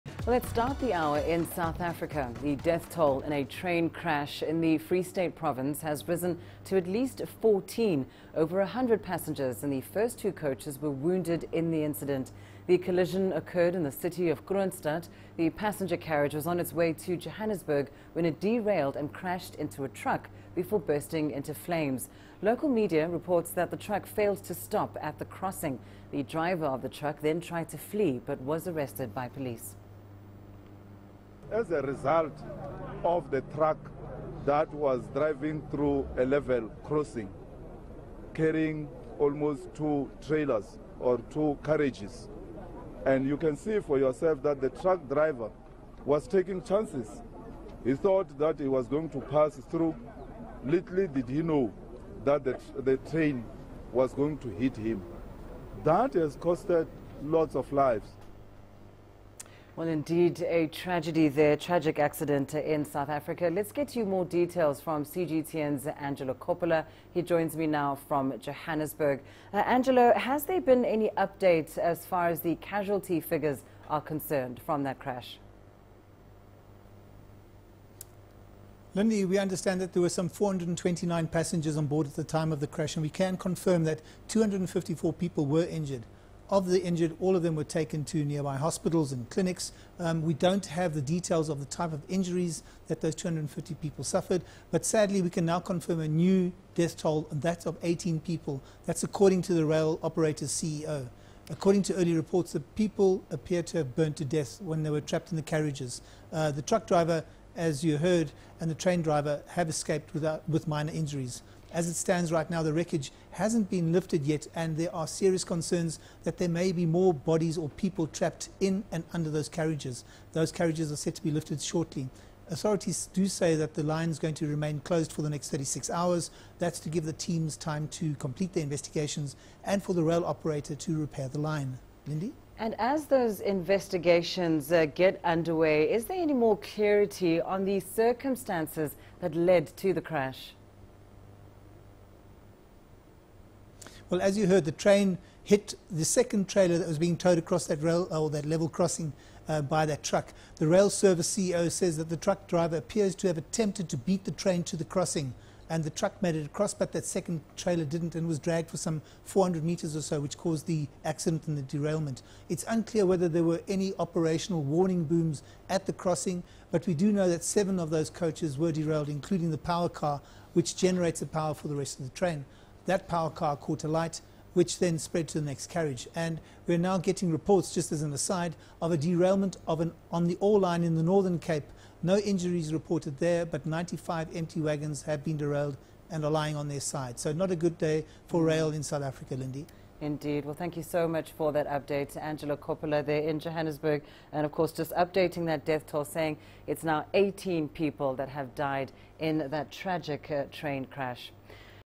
At least 14 killed and 180 injured in South Africa train crash
The death toll in a train crash in the Free State province has risen to at least 14. Over 100 passengers in the first two coaches were wounded in the incident. The collision occurred near Kroonstad city. The passenger carriage was on its way to Johannesburg  when it derailed and crashed into a truck before bursting into flames. Local media is reporting that the truck failed to stop at a crossing. The driver of the truck tried to flee, but was arrested by police.